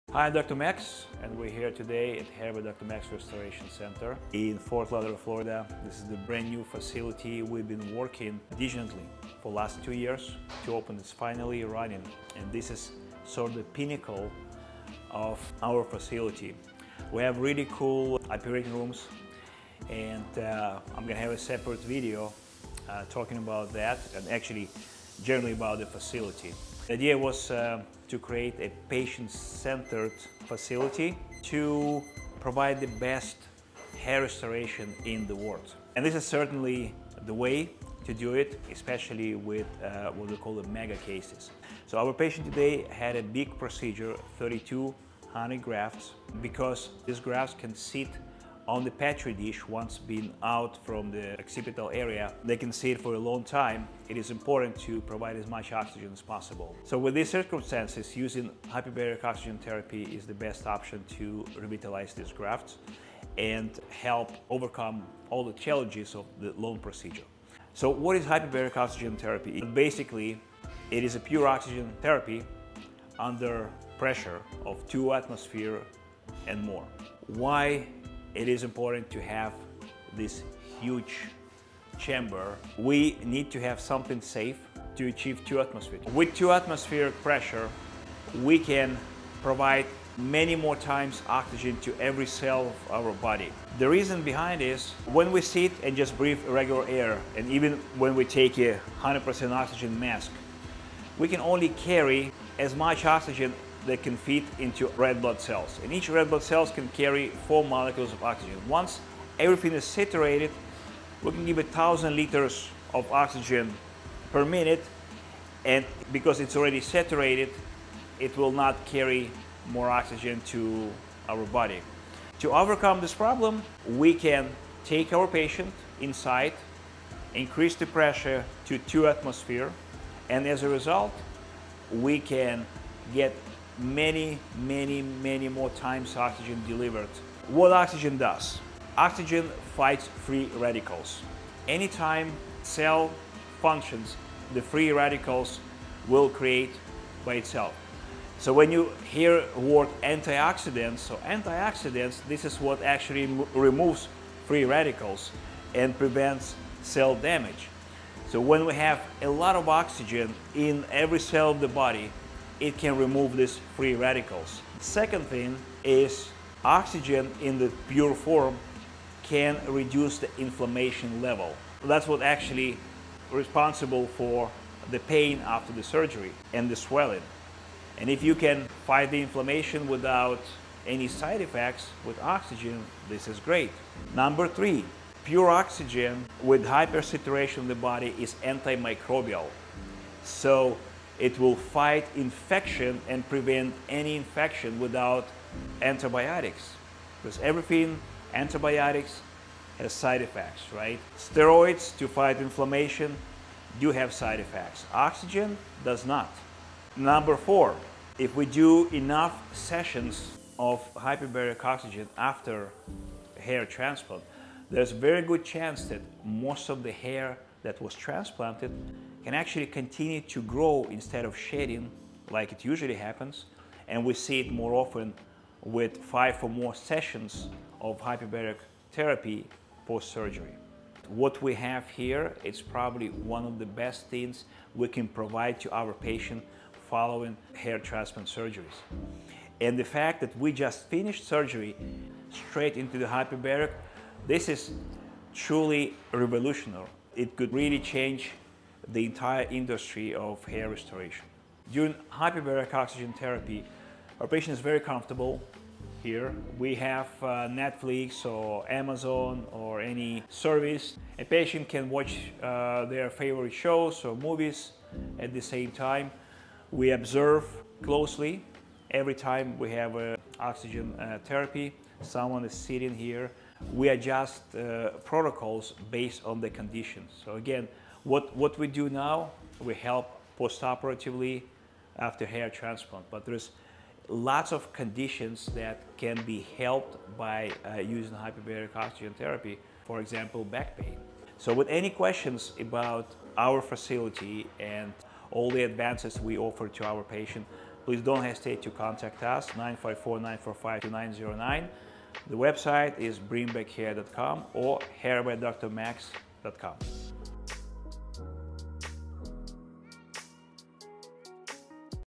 How does Hyperbaric Oxygen Therapy work?
Dr. Max Hyperbaric Therapy Center in Ft. Lauderdale Florida Hyperbaric oxygen therapy (HBOT) is a type of treatment used to speed up healing of carbon monoxide poisoning, gangrene, wounds that won't heal, and infections in which tissues are starved for oxygen. In this video, however, Dr. Max from Hair by Dr. Max Restoration Center highlights the benefits Hyperbaric Oxygen Therapy provides to patients who have just had a hair transplant procedure.

HBOT benefits include:
- Wound healing by bringing oxygen-rich plasma to tissue starved for oxygen. - Prevents "reperfusion injury"
- Helps block the action of harmful bacteria and strengthens the body's immune system
- HBOT encourages the formation of new collagen and new skin cells If you are interested in scheduling your hyperbaric oxygen therapy session, give us a call at